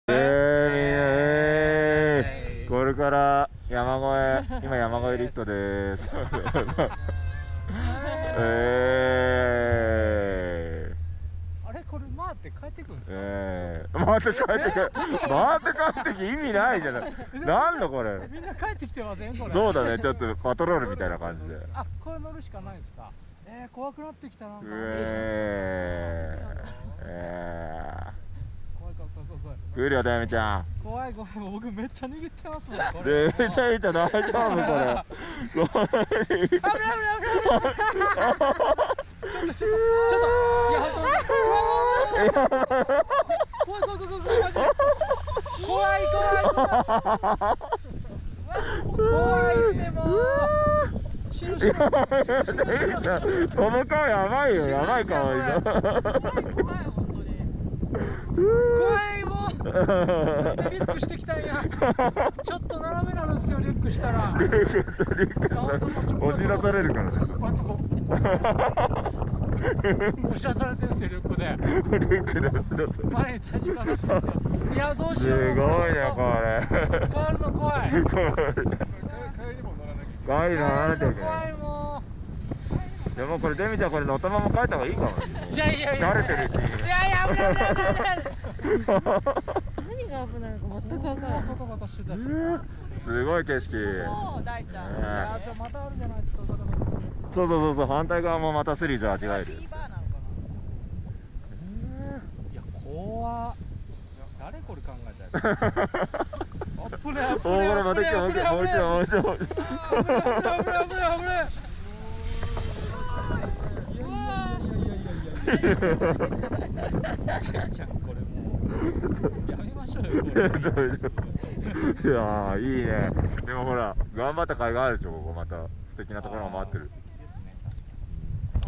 World's scariest ski lifts Val D'Isere Ski Resort "Leissieres Express" France 🇫🇷
Val D'Isere French 🇫🇷Ski Resort has World's scariest ski lifts Leissieres Express🇫🇷
Ski lifts go over the mountains.
After that, you will see one of the most amazing views of the French Alps.
Val D'Isere & Tignes Ski Resort (ESPACE KILLY) is one of the best ski resort in France.

フランス🇫🇷Val D'Isere　ヴァルディゼール　スキー場（ティーニュスキー場と合わせてエスパスキリーと呼ばれてます）にある、山を越える世界で一番恐ろしいスキーリフト"Leissieres Express"
山を越えれば、そこには海外の広くて壮大なスキー場の絶景が！
ヴァルディゼールとティーニュスキー場はフランスで最高なスキー場の一つです。一つのリフト券で二つのスキー場を滑れます。大きくて1日ではとても滑りきれません。
オフピステエリアは10,000ヘクタール